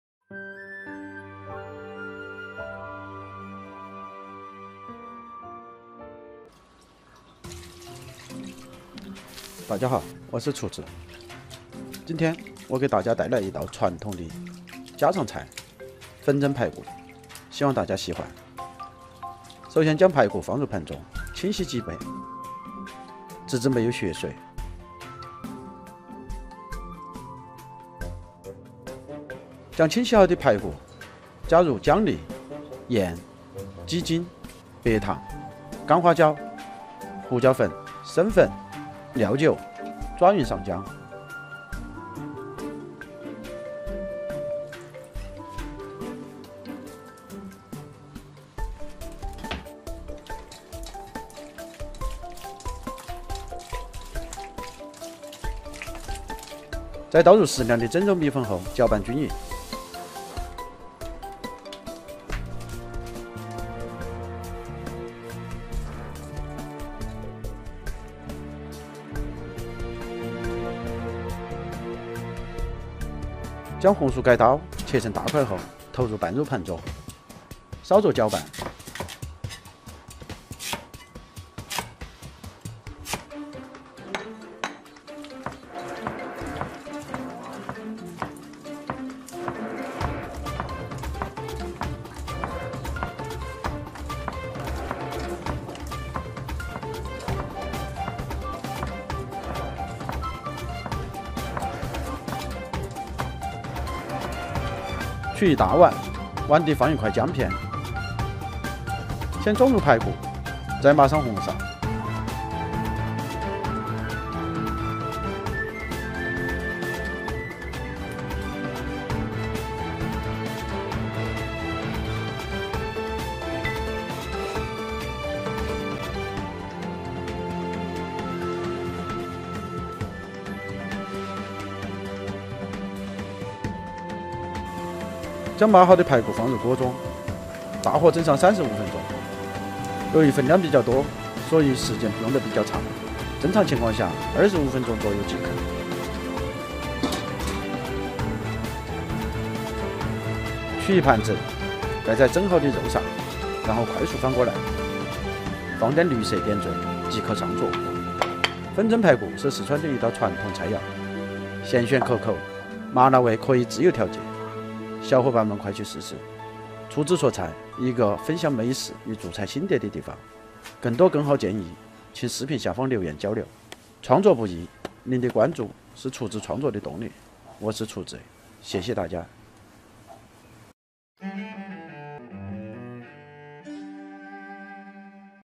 [Chinese food] It takes only 3 minutes for a video to cook for almost an hour.
Chinese food is famous all over the world, but you may be shocked by its surprising range and variety of ingredients if you've only eaten in Chinese